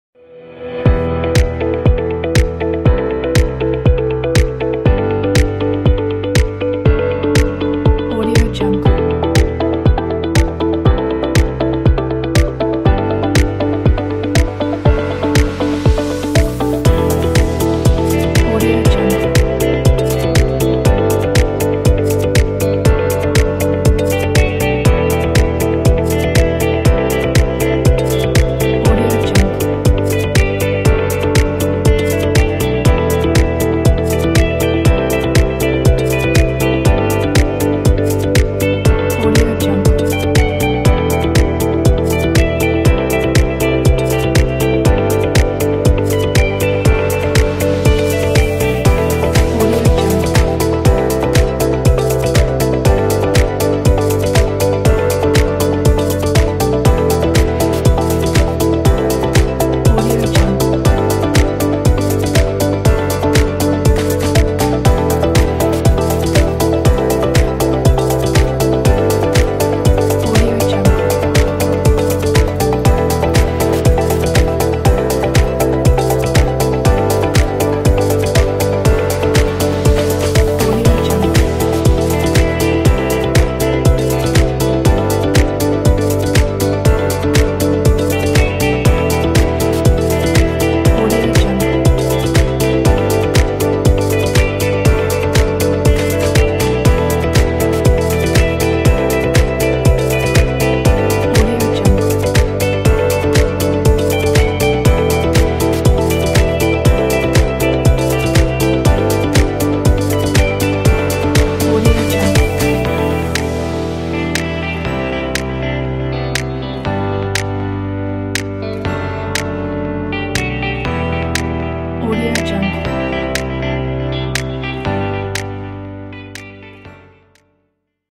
Phone Presentation | After Effects Template
Phone Presentation perfect for your iOS app or mobile version of website promo video.
Project Features:
After Effects CS5 and above compatibility
FULL HD (1080p) HD (720p) / 30 fps
Black / white / golden colors version of phone
Every scene has a unique animation of phone
Multi-purpose
Clean design
Modular structure
19 media placeholders
Fast rendering
Effects control panel – adjusting one click
Link to free font is included
Detailed Help file is included
Soundtrack
Support:
Screenshots: